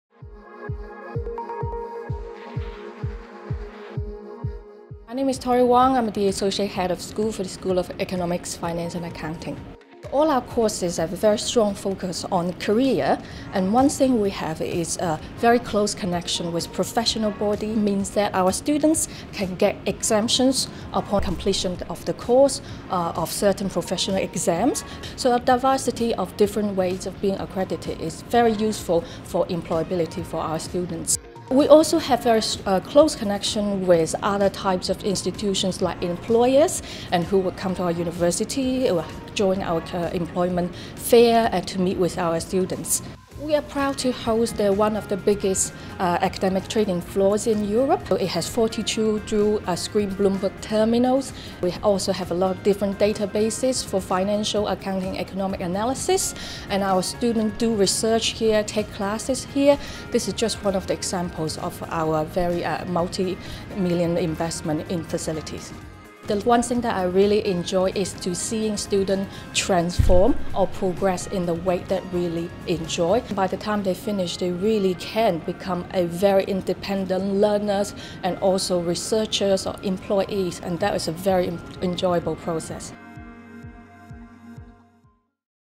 Head of School Torri Wang, welcomes you to the School of Economics, Finance and Accounting.
Torri Wang introduces you to some of the facilities, and what you can expect when you join Coventry University.